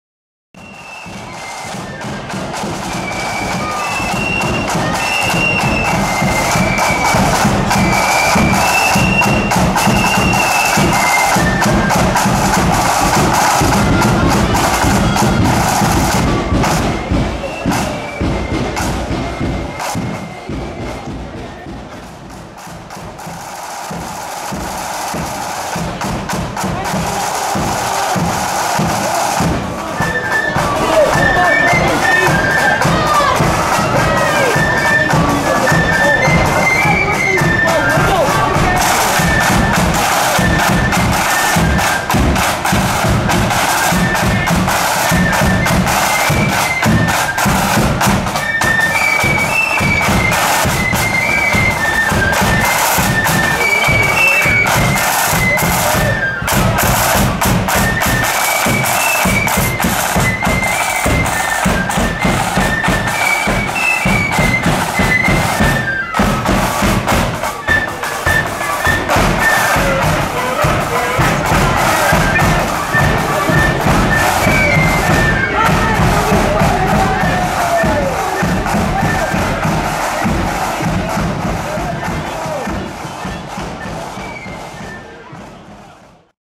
Moneydig Young Conquerors @ Portrush Sons of Ulster Parade 2018
Moneydig Young Conquerors on the march at Portrush Sons of Ulster annual band parade Saturday 14th July 2018